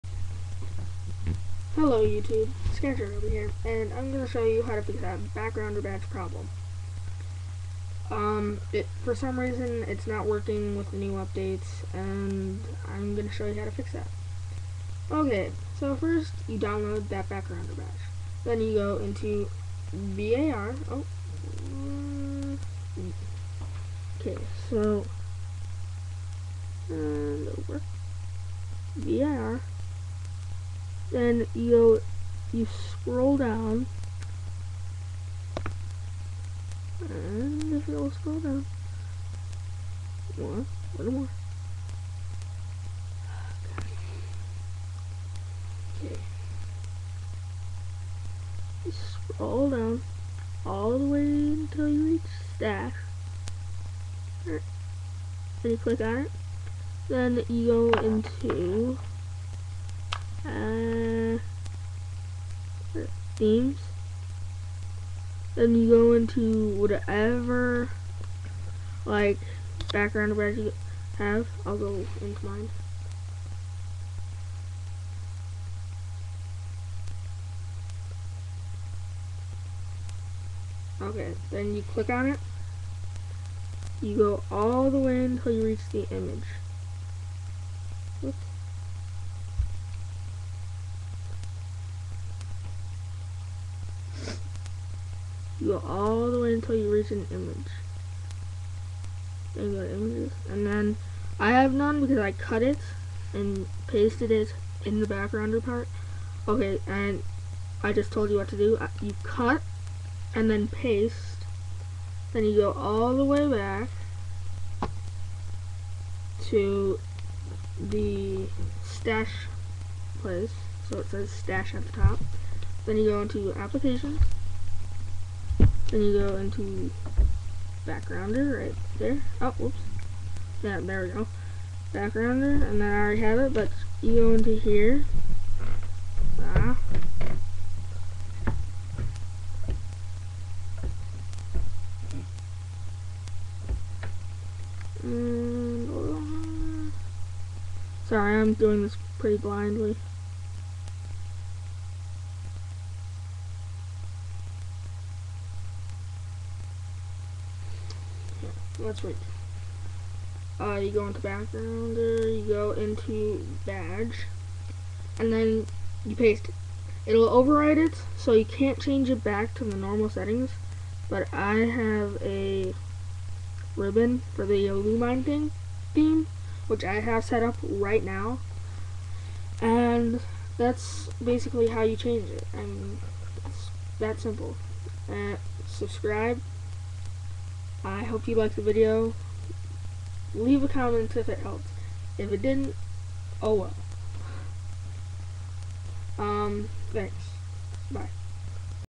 how to fix backgrounder badge problem!!!
This is a video on how to fix the backgrounder badge update so for all you cydiasters i would watch this